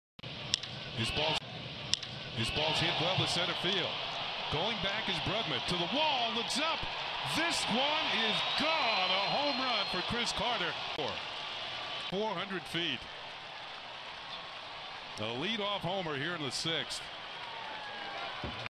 Chris Carter belts a solo home run to center
Chris Carter hammers a solo home run to center field to give the Yankees a 5-4 lead in the top of the 6th inning